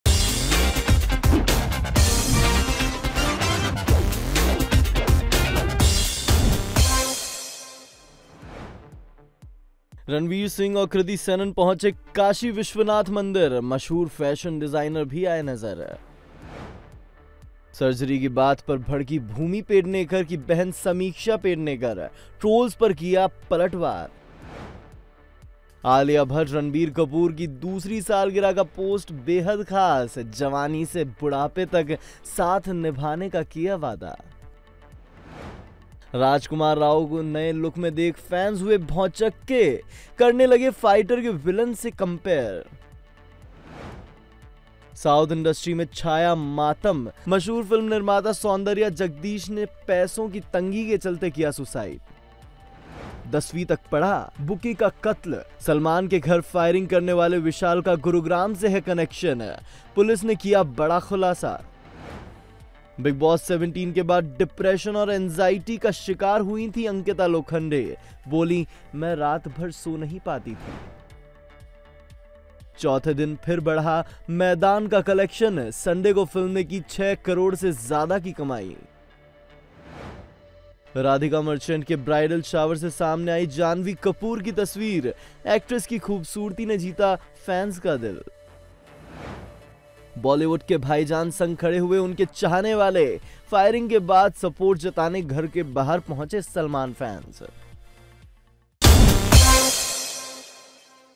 Surgery की बात पर भड़कीं Samiksha Pednekar, Salman Khan के Fans ने दिया हौंसला| Entertainment Top 10
Ranveer Singh और Kriti Sanon पहुंचे Kashi Vishwanath Mandir। Rajkumar Rao को New Look में देख फैंस हुए भौचक्के। South Industry के मशहूर Film Producer soundarya jagadish ने किया सुसाइड। Janhvi Kapoor की खूबसूरती ने जीता फैंस का दिल। 'Bigg Boss 17' के बाद डिप्रेशन का शिकार हुई थीं Ankita Lokhande।